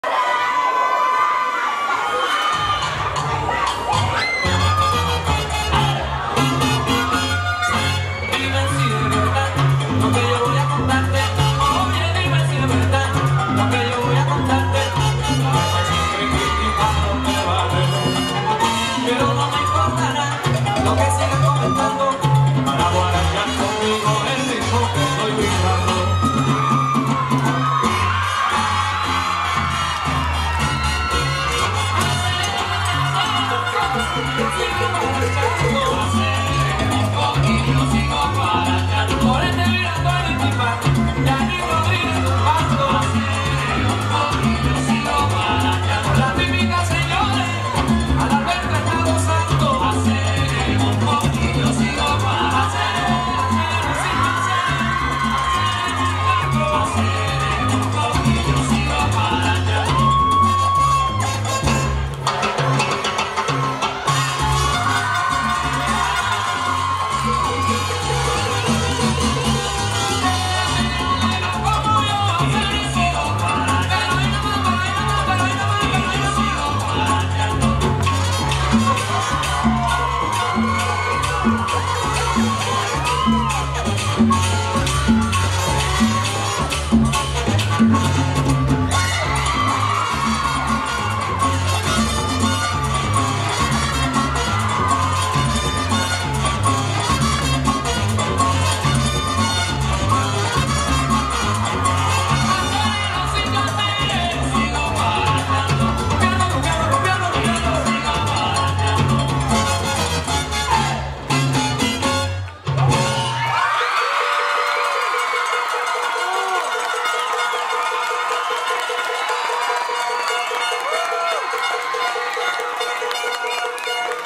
DANCE EXTRAVAGANZA 2019 /MELANY & KEVIN
MESTIZO ART CENTER
DANCE EXTRAVAGANZA 2019
SALSA JUNIOR  PAREJA
MELANY AND KEVIN
FIRTS PLACE
MESTIZO ART CENTER